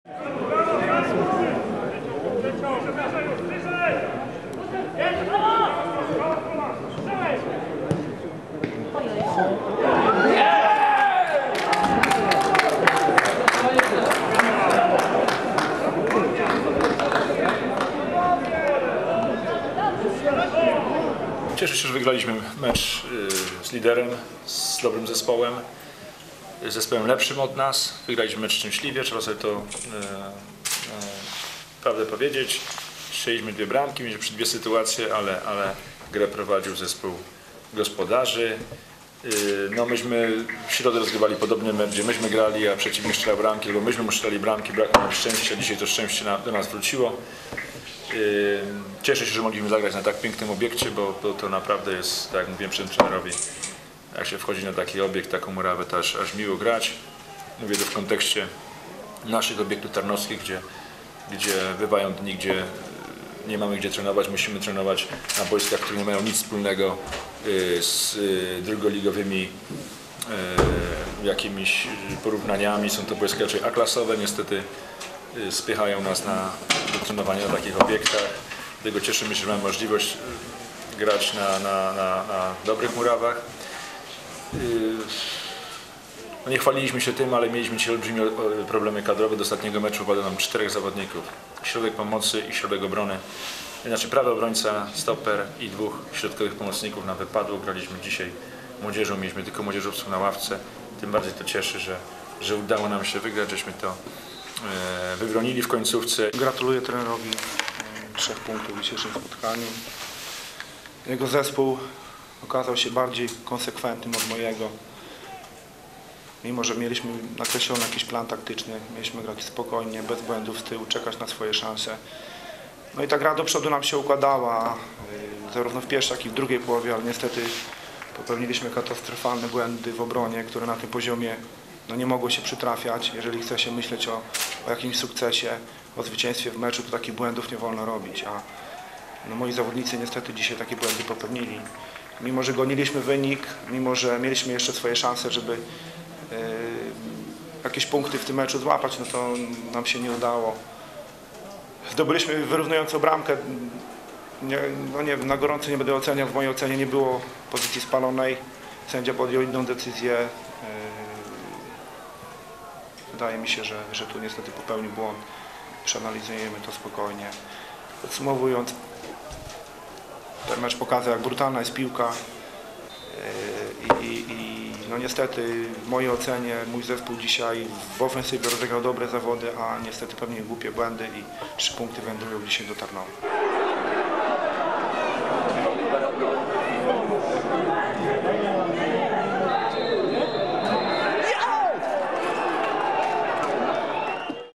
Wisła Puławy - Unia Tarnów 1-2 (skrót, wypowiedzi)
WISŁA PUŁAWY - UNIA TARNÓW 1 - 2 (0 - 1)

0-1 Wróbel 25, 0-2 Wróbel 70, 1-2 Martuś 87.

Wisła: Peńkoweć - Gawrysiak, Kursa, Skórnicki, Budzyński - Pożak (60 Martuś), Maksymiuk, Więcek (69 Honczaruk), M.Nowak (64 Charzewski) - Nowosielski, K. Nowak.
Unia: Libera - Witek, Pawlak, Bednarczyk, Węgrzyn - Radliński, Ślęzak (82 Szczerba), Drozdowicz, Sojda (72 Wilk) - Fałowski (88 Adamski), Wróbel.

Sędziował: Jacek Kikolski z Płocka.
Żółte kartki: Drozdowicz, Witek, Radliński.
Widzów: 700